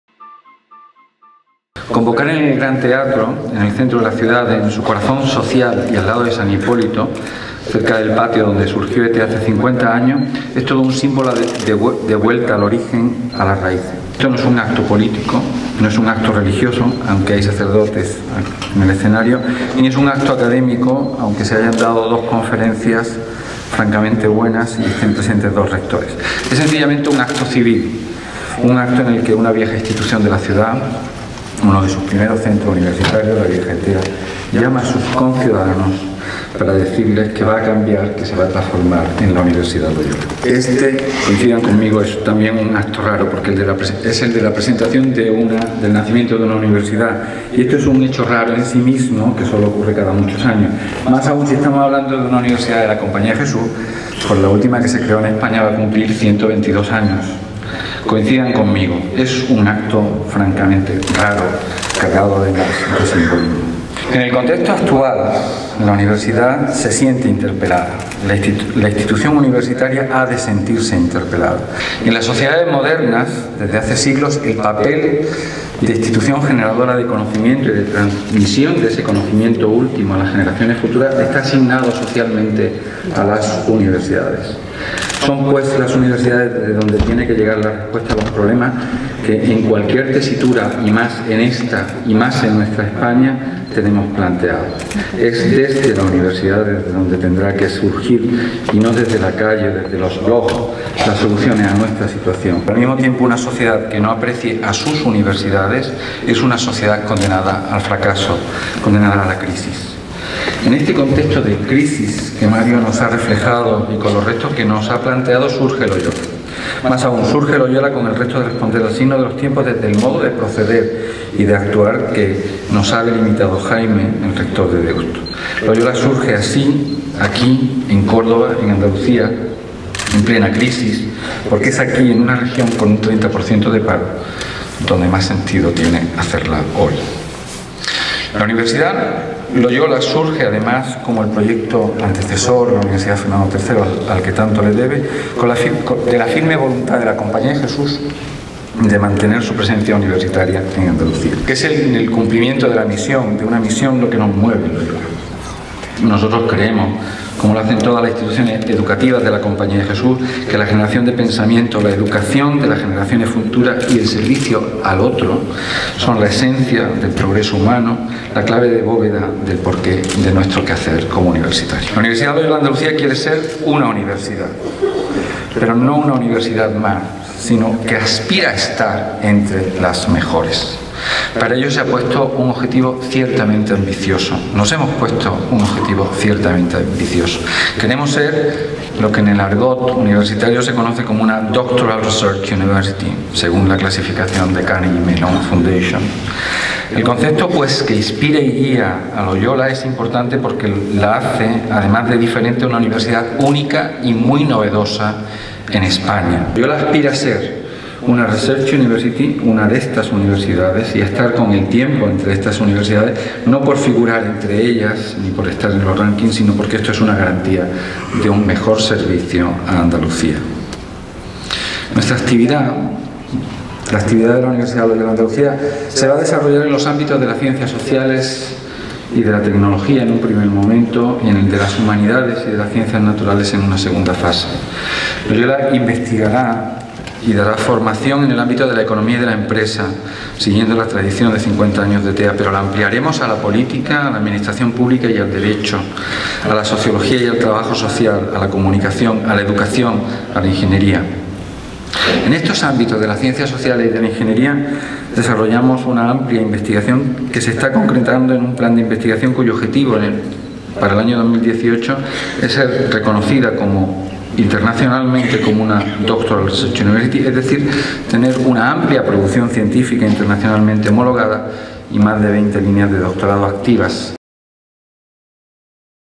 Acto inaugural Universidad Loyola Andalucía (CORDÓPOLIS, Víctor Molino)
El Rector de la Universidad Loyola Andalucía, Gabriel Pérez Alcalá, presenta a la sociedad cordobesa el nuevo proyecto docente de la Compañía de Jesús.